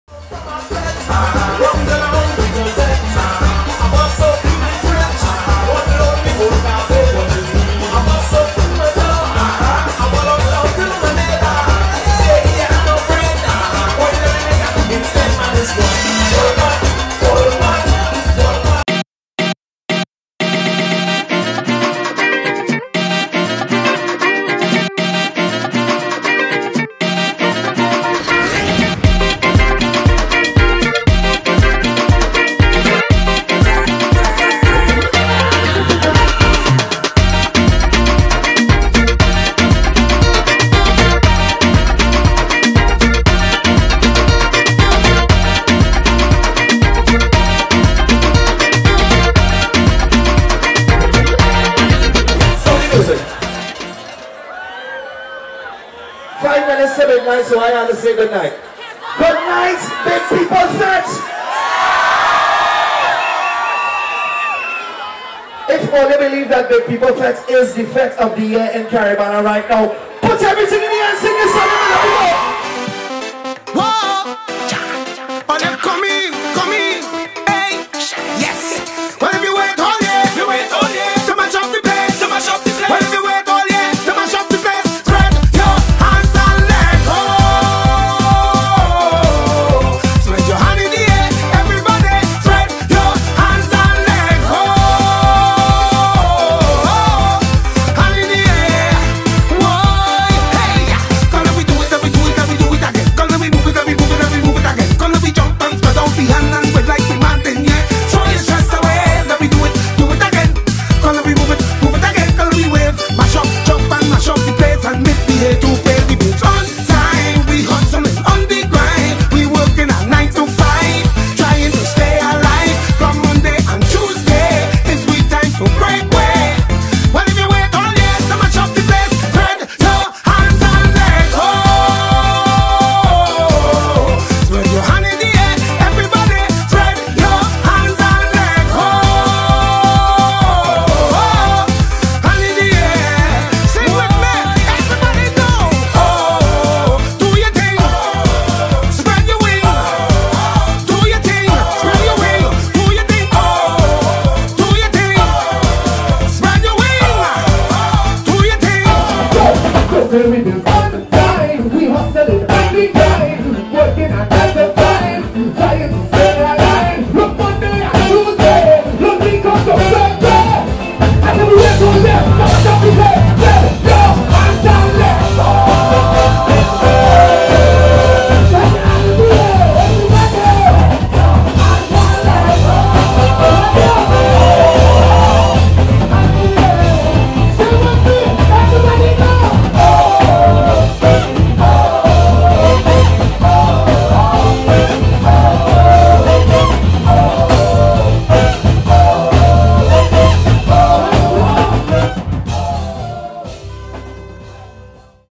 BIG PEOPLE FETE 2013 Recap Video
Simply put, this is the most anticipated event of Toronto Carnival weekend.